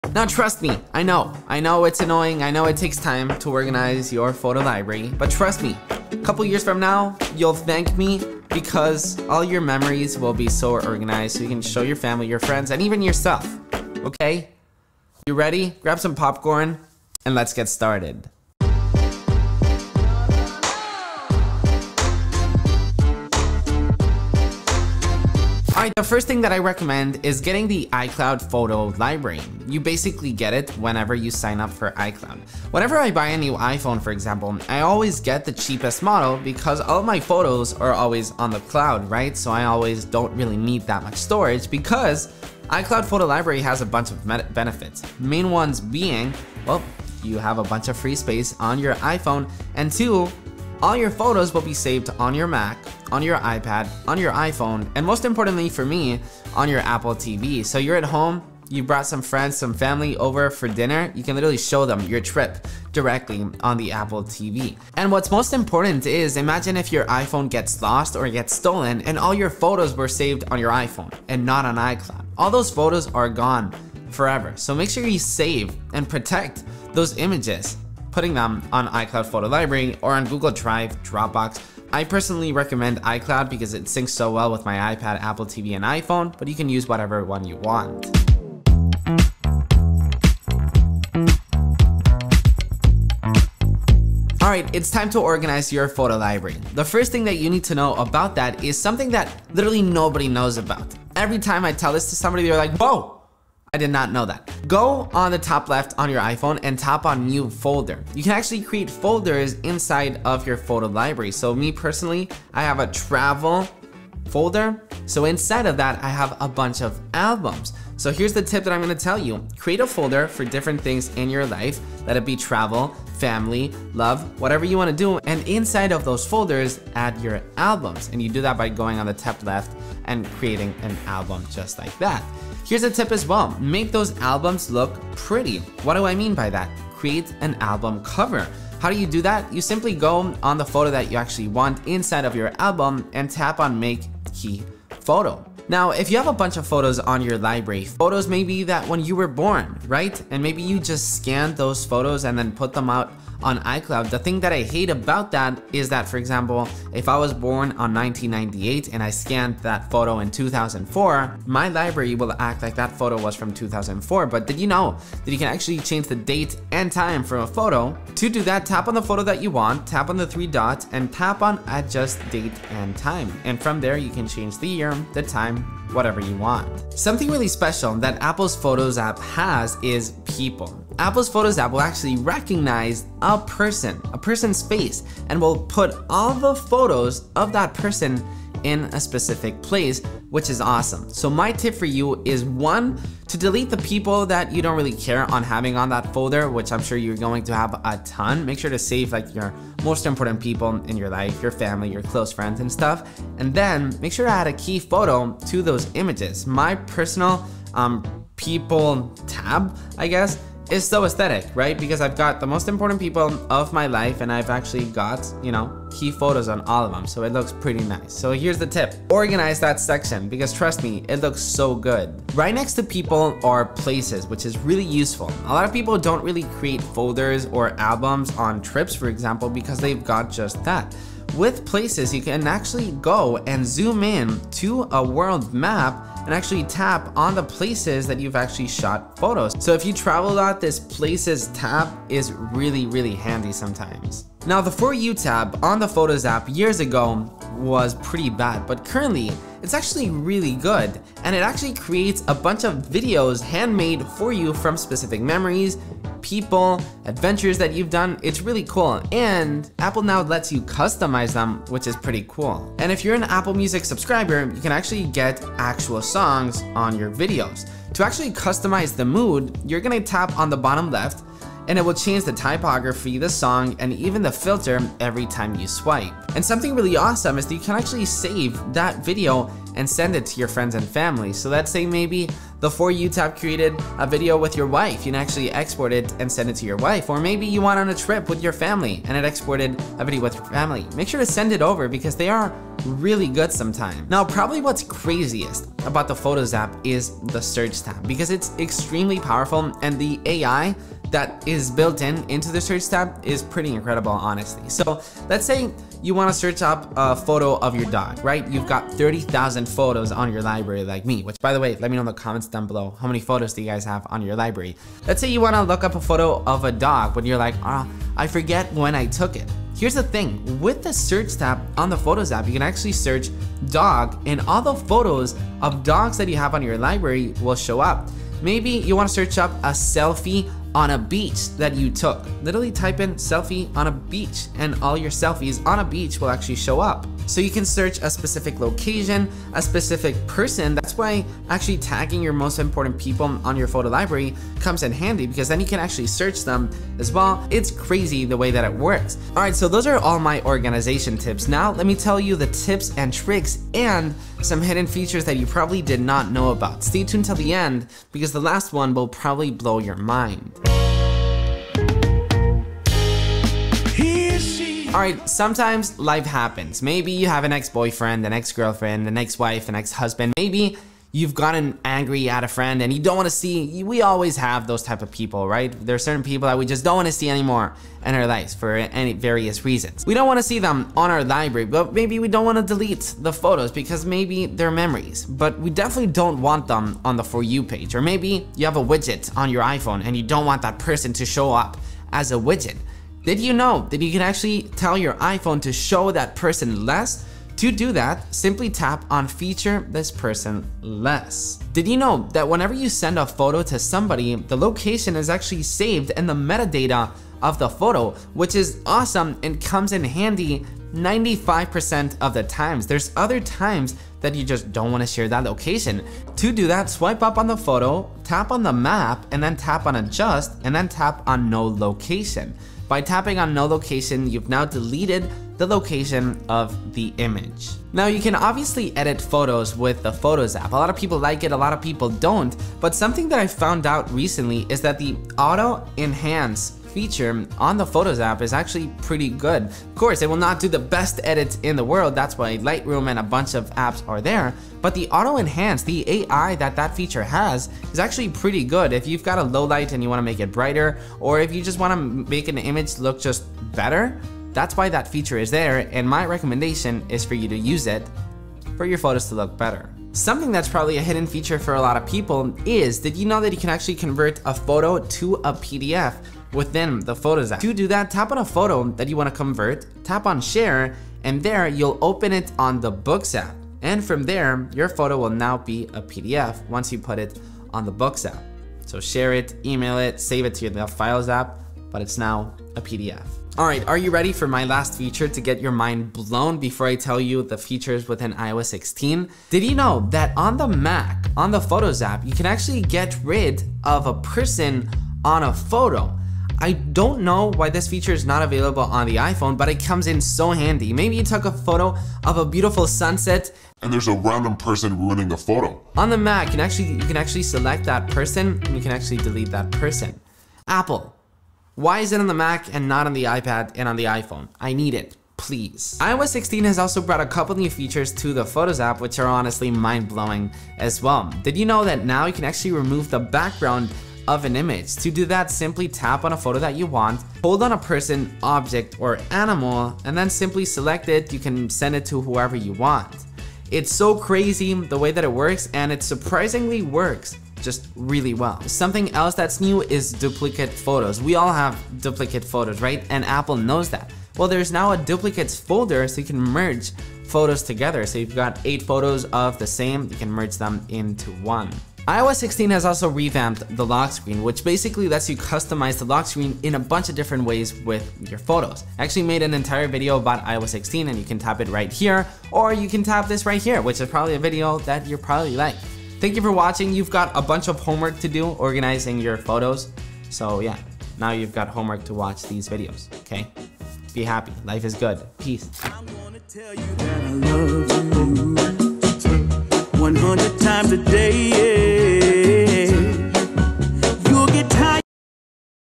Apple Photos - how to make it (really) useful.
The photos app on our iPhone, iPad and Mac is the home for all of our memories. That's why in this video, I'll tell you the most useful tips and tricks to take the full potential of what the app has to offer. From editing photos to organizing techniques.

TIME STAMPS
00:00 hello :)
00:26 iCloud Photo Library
01:29 Add Folders
02:15 Album Covers
02:30 Adjust Date & Time
02:57 People
03:40 Places
04:05 For You
04:55 Search
05:50 Tips & Tricks
06:05 Hide People
06:42 Remove Location
07:08 Auto Edit
07:40 Convert to PDF
08:03 Remove Object on Photo
08:35 New iOS 16 Features
08:40 Remove Background
08:59 Duplicate Photos
09:12 Lock Screen